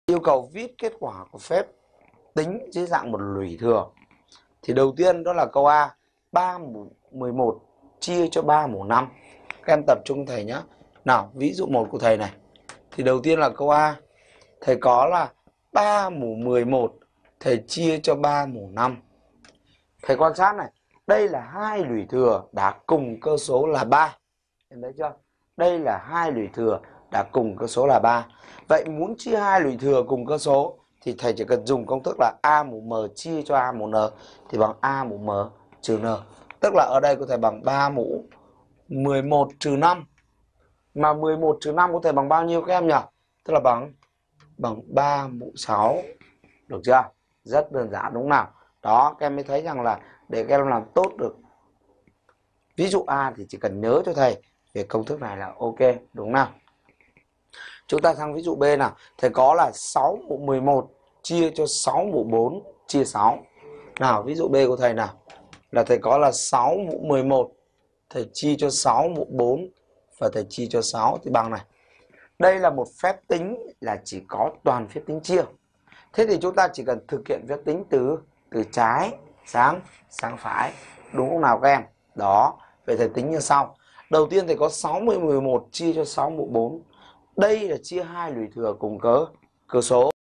Chia hai luỹ thừa cùng cơ số - Ôn tập và bổ túc số tự nhiên - Toán 6
Ví dụ 1: Thực hiện phép tính
a) 12.4−24:4+12
b) 4^2.2 − 36 : 3^2
c) 21 + 6^2:4 − 2.3^2
d) 5.2^2 + 4.2^2 − 4^2.1^7
Học online bài
THỨ TỰ THỰC HIỆN CÁC PHÉP TÍNH